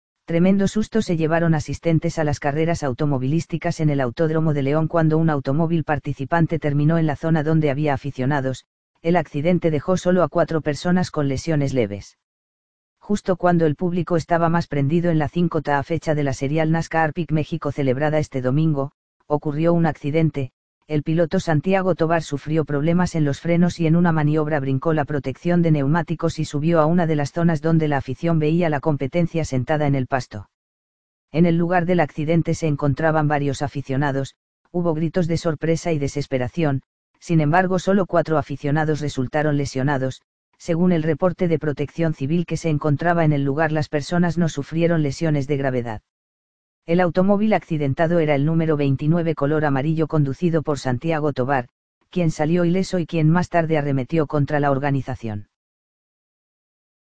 Cuatro lesionados en accidente dentro del Autódomo de León!
Tremendo susto se llevaron asistentes a las carreras automovilísticas en el Autódromo de León cuando un automóvil participante terminó en la zona donde había aficionados, el accidente dejó sólo a cuatro personas con lesiones leves.

Justo cuando el público estaba más prendido en la 5ta fecha de la Serial Nascar Peak México celebrada este domingo, ocurrió un accidente, el piloto Santiago Tovar sufrió problemas en los frenos y en una maniobra brincó la protección de neumáticos y subió a una de las zonas donde la afición veía la competencia sentada en el pasto.

En el lugar del accidente se encontraban varios aficionados, hubo gritos de sorpresa y desesperación, sin embargo sólo cuatro aficionados resultaron lesionados, según el reporte de Protección Civil que se encontraba en el lugar las personas no sufrieron lesiones de gravedad. El automóvil accidentado era el número 29 color amarillo conducido por Santiago Tovar, quien salió ileso y quien más tarde arremetió contra la organizacion.